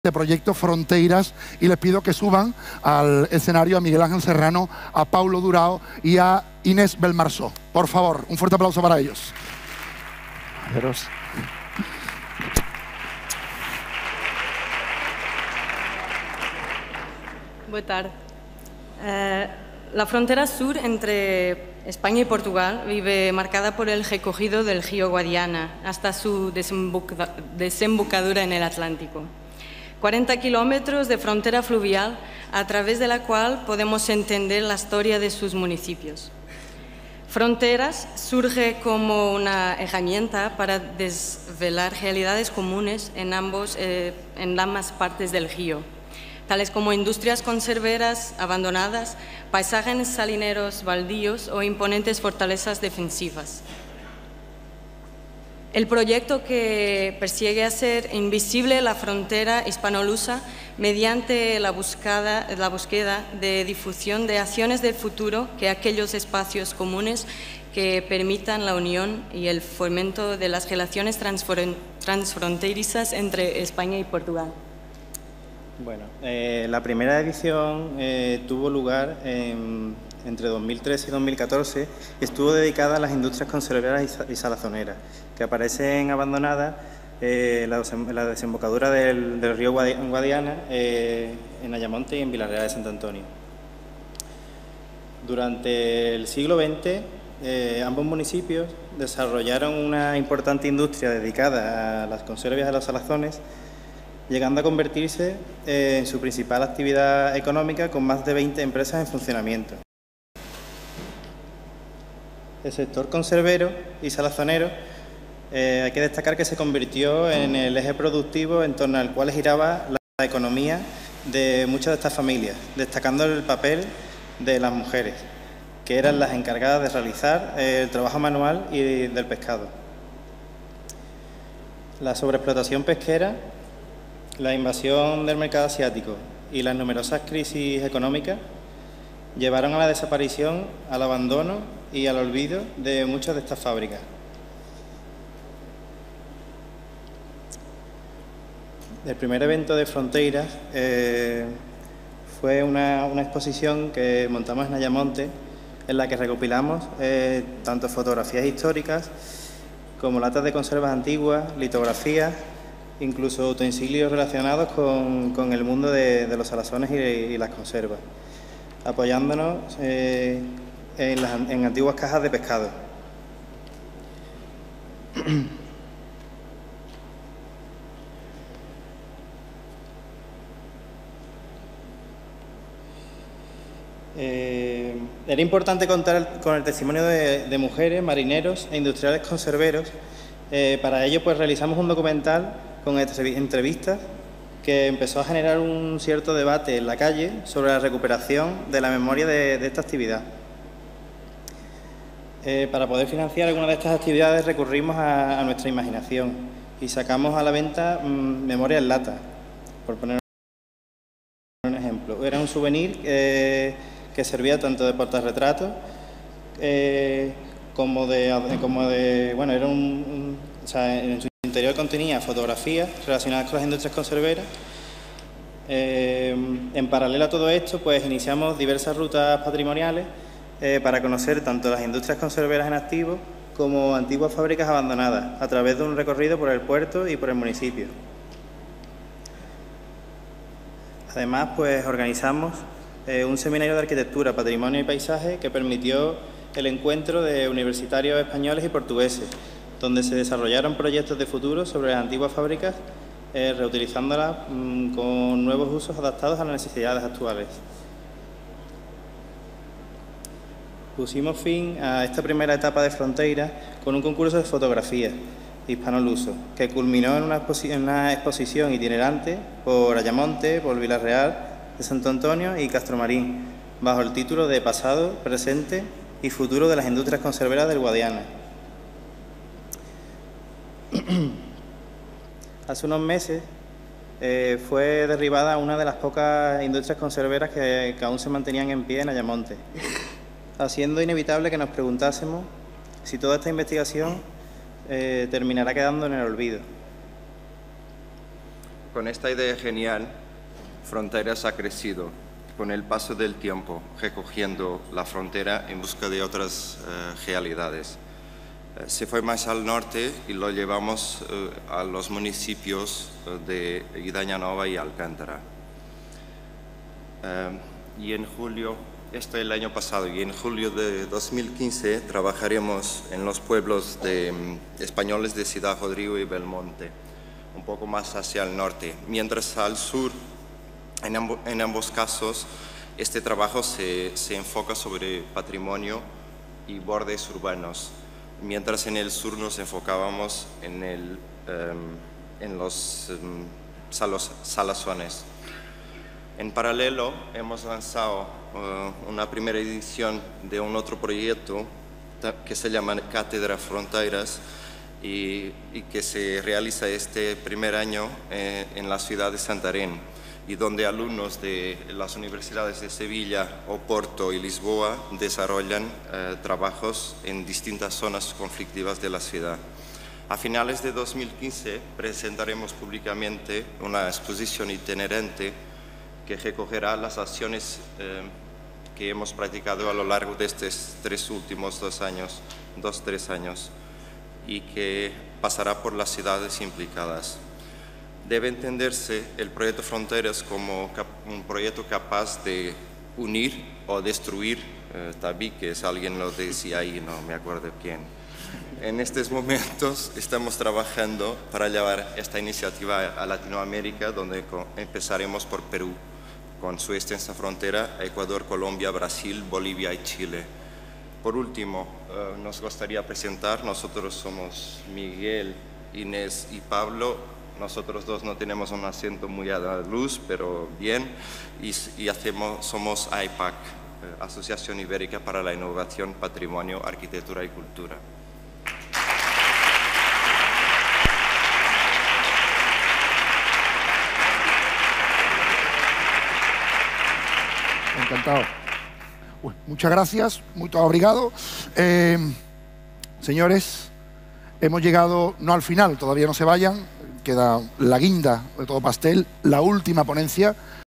FRONTEiRAS _ PKN Sentido Sur
Presentación del proyecto FRONTEiRAS el pasado 26 de marzo de 2015 en el formato PKN de "SENTIDO SUR: Fórum Internacional de Turismo y Personas Emprendedoras del Sur de Europa"